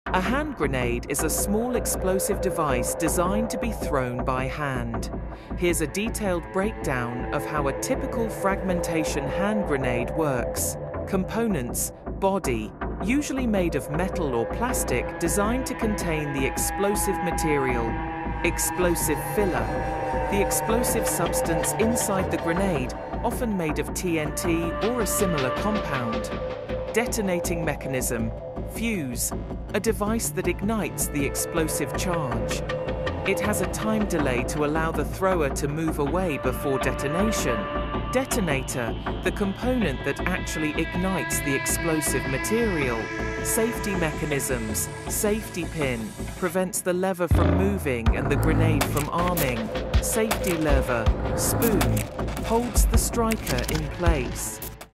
How Hand Grenade Works
A hand grenade is a small explosive device designed to be thrown by hand. Here's a detailed description of how it works:

Components of a Hand Grenade
Body: Typically made of metal or plastic, containing the explosive material.
Fuse: A timing device that delays the explosion after the grenade is thrown.
Safety Pin and Lever (Spoon): Safety mechanisms to prevent accidental detonation.
Detonator: An initiator for the explosive material.
How a Hand Grenade Works
Preparation: The user holds the grenade, keeping the safety lever (spoon) depressed. The safety pin is pulled out, but the grenade does not explode as long as the lever is held.

Arming: When the grenade is thrown, the safety lever is released. This action allows a spring-loaded striker to hit a primer, igniting the fuse.

Fuse Ignition: The fuse burns for a predetermined amount of time, usually between 2 to 5 seconds. This delay allows the thrower to get clear of the blast radius.

Detonation: Once the fuse burns out, it triggers the detonator, setting off the main explosive charge within the grenade body.

Explosion: The explosive charge detonates, causing the grenade body to fragment. These fragments are propelled outward at high speed, creating a lethal radius.